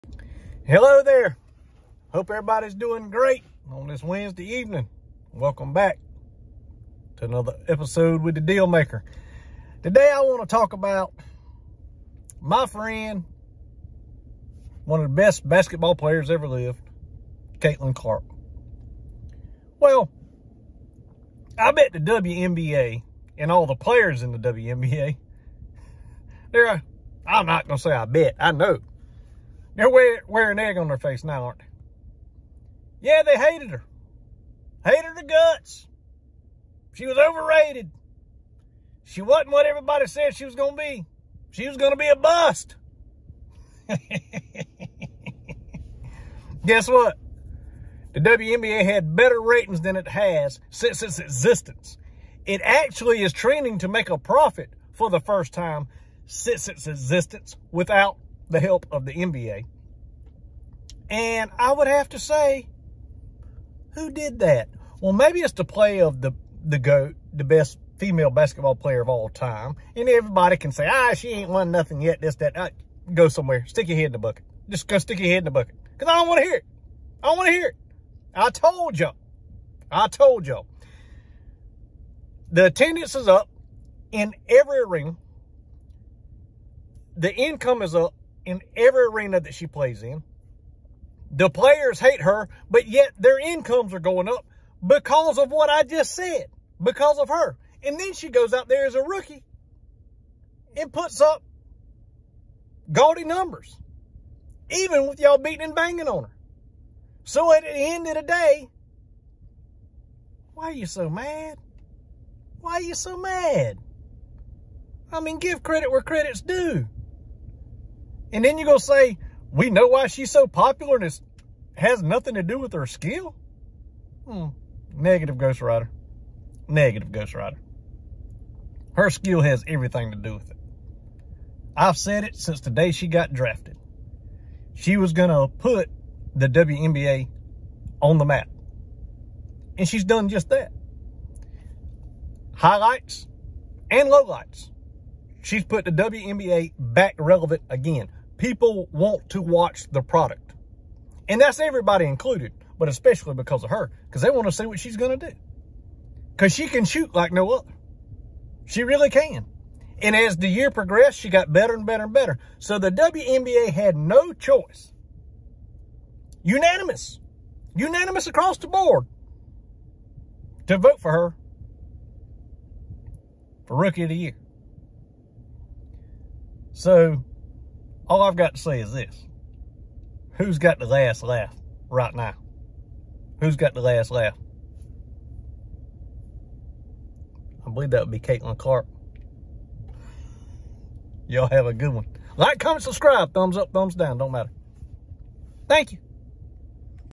Caitlin Clark gets the last Laugh at all her Haters and WNBA Players!!! 😂
After putting the WNBA on the map and making them relevant. Then winning Rookie of the Year, I believe respect is now Due!!